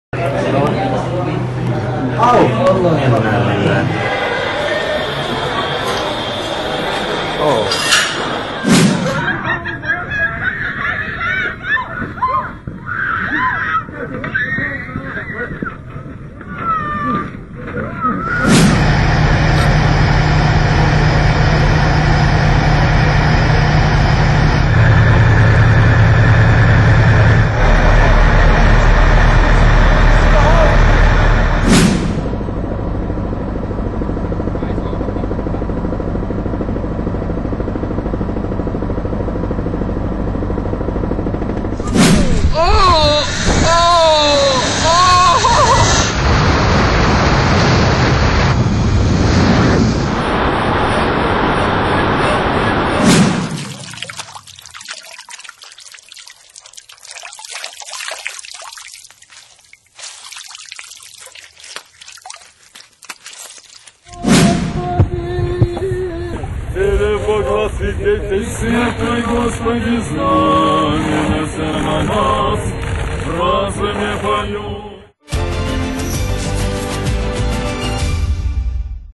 بدون شرح هفته؛ از ریزش پل در کلمبیا تا پریدن یک پنگوئن روی قایق محققان…
در یک ویدئوی پربیننده دیگر یک پنگوئن آدلی به روی قایق محققان قطب جنوب پرید. او بعد از بررسی اطراف خود دوباره به درون آب رفت. …